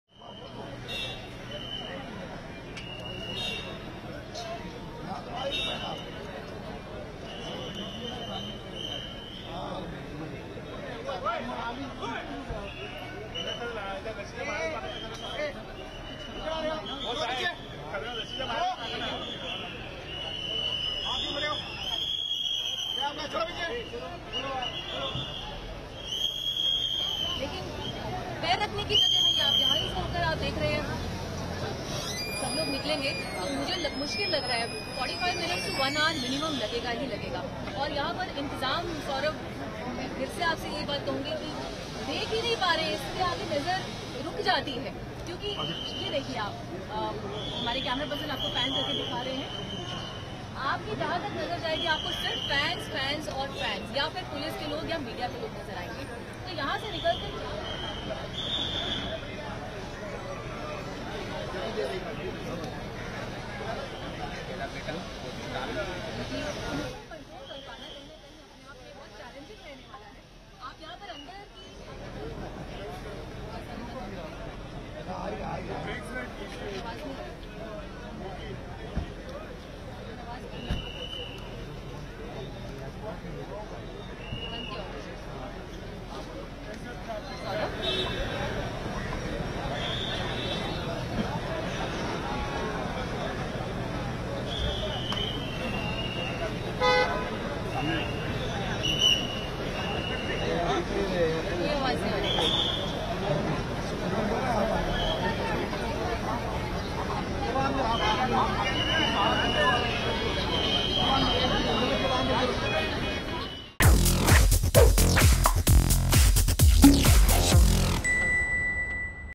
Adieu, Sridevi! Colleagues, Fans Arrive to Pay Last Respects
Actor Sridevi’s body has been kept at the Celebration Sports Club in Lokhandwala,Mumbai, for well-wishers to pay their last respect to the Bollwyood icon.